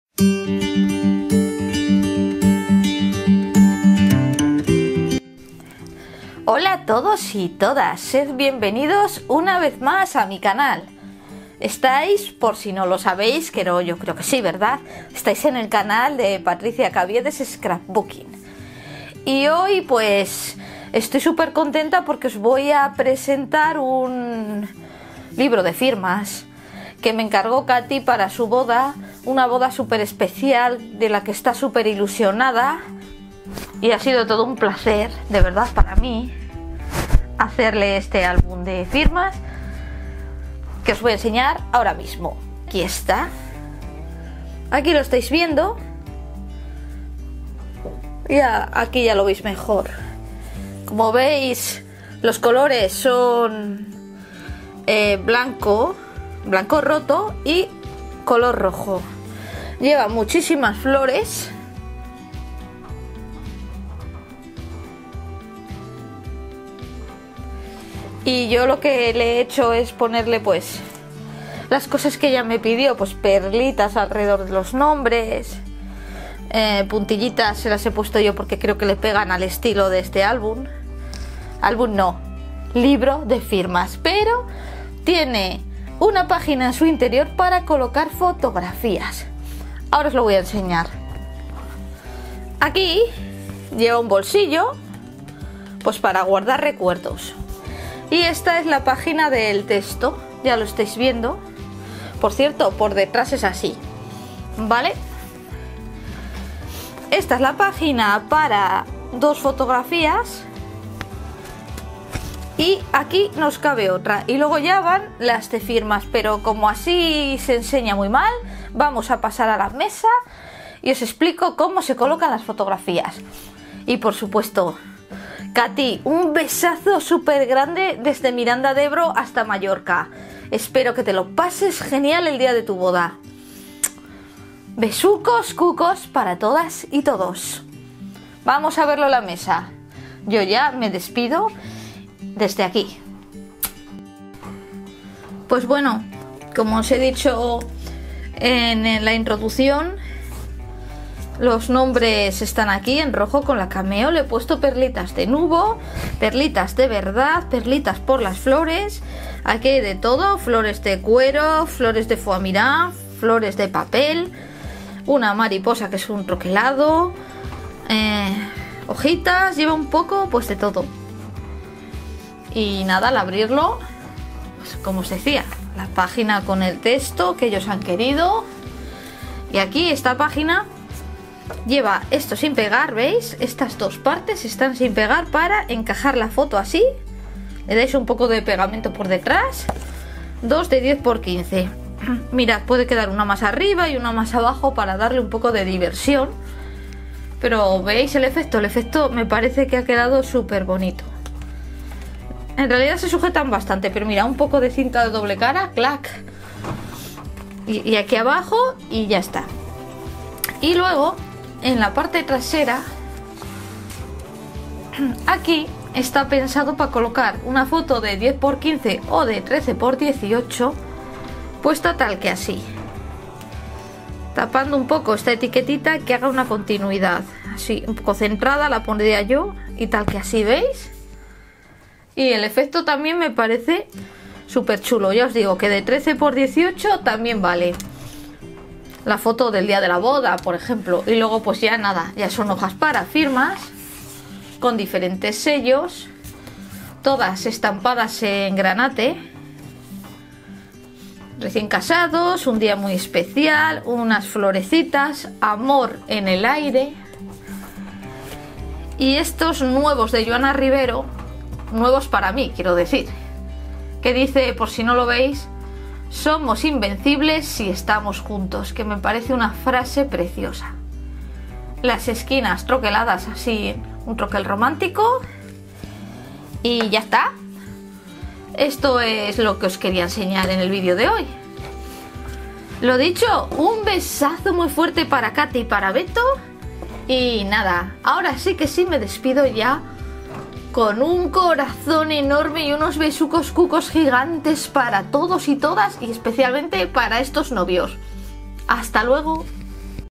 libro de firmas scrap boda Katy&Beto
¡Hace mucho que no nos vemos la cara! Hoy os enseño este libro de firmas para boda que ha viajado hasta Mallorca.